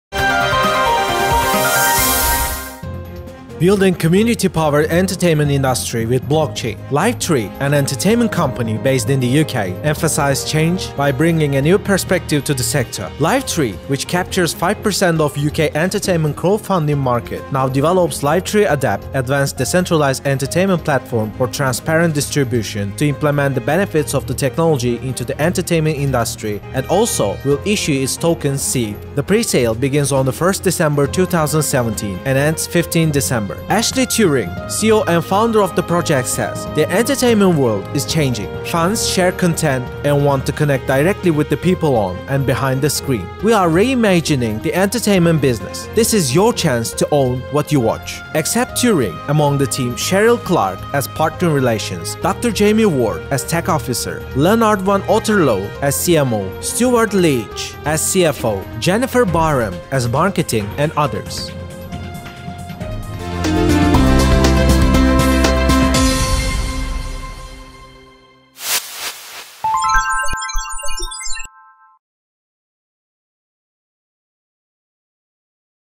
Entertainment through advanced decentralized platform
Building community-powered entertainment industry with blockchain. LiveTree, an entertainment company, based in the UK, emphasizes change by bringing a new perspective to the sector. LiveTree, which captures %5 of UK entertainment crowdfunding market, now develops LiveTree ADEPT (Advanced Decentralized Entertainment Platform for Transparent distribution), to implement the benefits of the technology into the entertainment industry, and also will issue its tokens ‘’Seed’’, the pre-sale begins on 1 December 2017, and end 15 December. Ashley Turing, CEO and founder of the project says: ‘’The entertainment world is changing. Fans share content and want to connect directly with the people on and behind the screen. We are reimagining the entertainment business. This is your chance to own what you watch.’’ Except Turing, among the team; Cheryl Clarke, as partner relations; Dr. Jamie Ward, as tech officer; Lennard van Otterloo as CMO; Stuart Leitch, as CFO; Jennifer Barham, as marketing, and others.